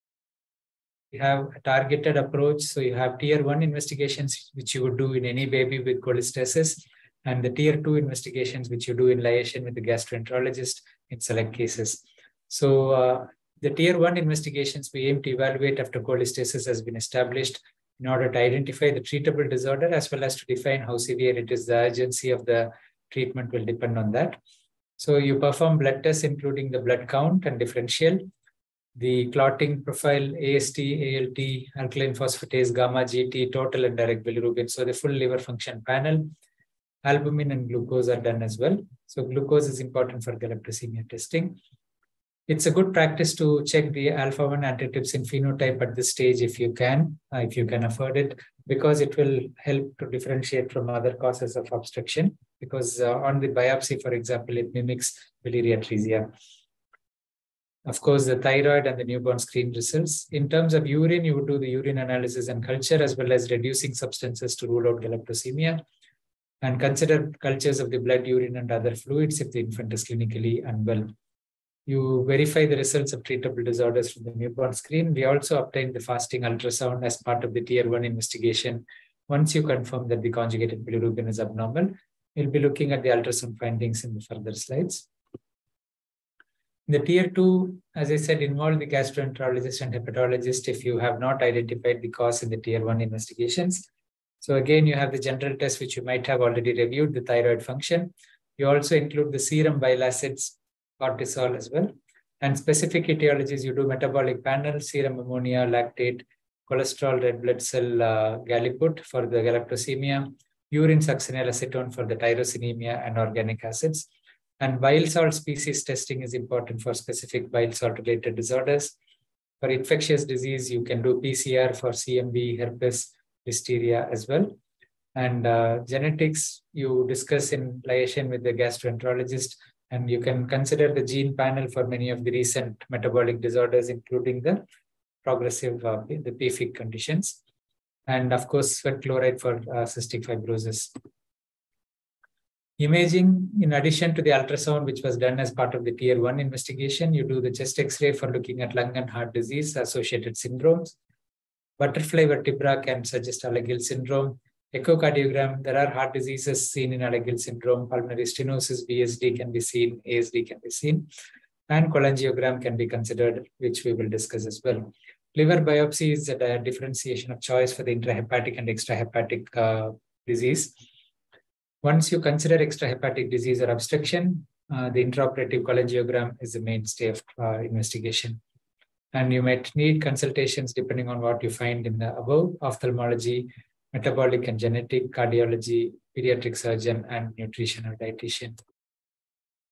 Approach to Neonatal cholestasis-tiered approach to investigations
Neonatal jaundice is a common problem, but when jaundice is prolonged, we need to rule out conjugated jaundice (suggestive of cholestasis)-this is a serious problem as there is a risk of conditions like biliary atresia that need urgent treatment. In this video, I discuss the tiered approach to work up and investigations.